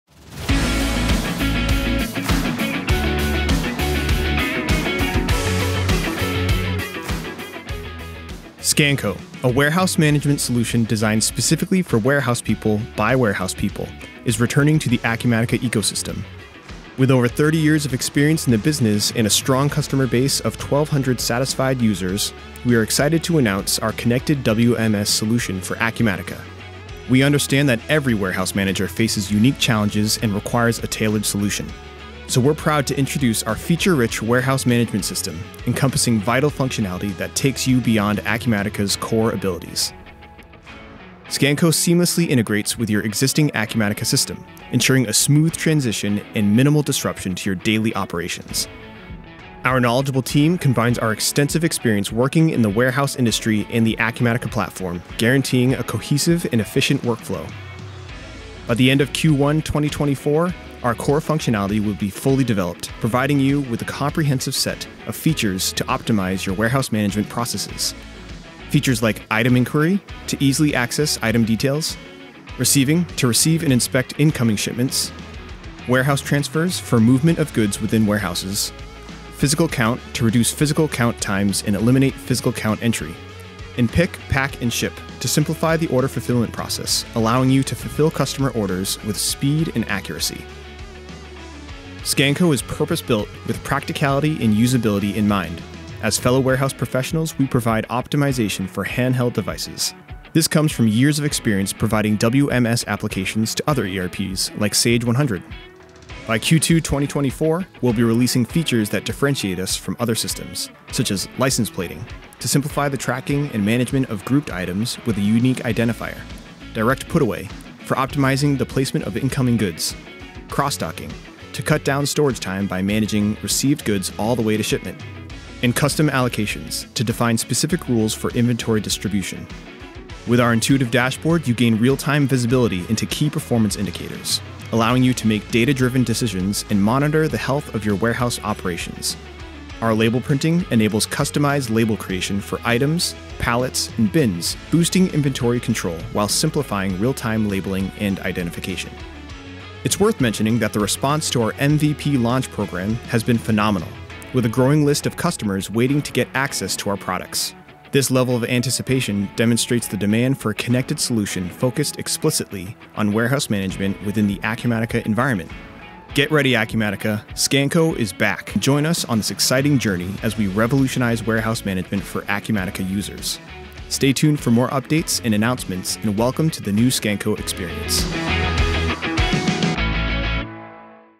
Scanco (WMS) Warehouse Management Solution for Acumatica
We have designed Scanco to seamlessly integrate with your existing Acumatica system, ensuring a smooth transition and minimal disruption to your operations. Unlike the core Acumatica product, which may lack optimization for handheld devices and fail to cater to the unique needs of warehouse managers, Scanco is purpose-built with practicality and usability in mind.